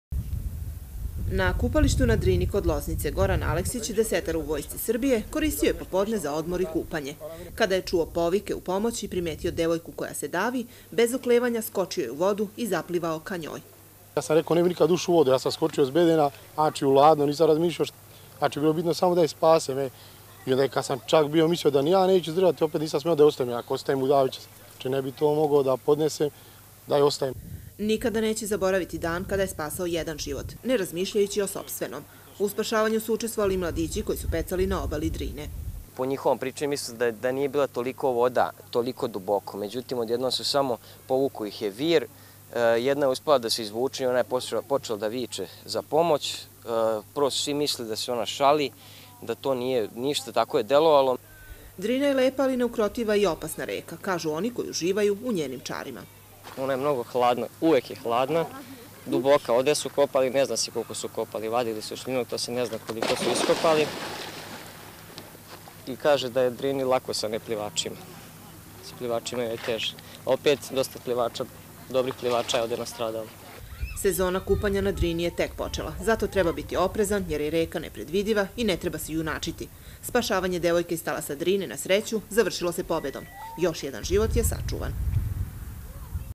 Heroj sa Drine
Neopreznost, ali i nepredvidiva Drina, zamalo su koštale života jednu mladu Lozničanku. Srećom, desetar Goran Aleksić bio je u blizini i spasio je. Pomogli su i mladići koji su pecali na obali reke. Copyright : Javni Servis Srbije (RTS)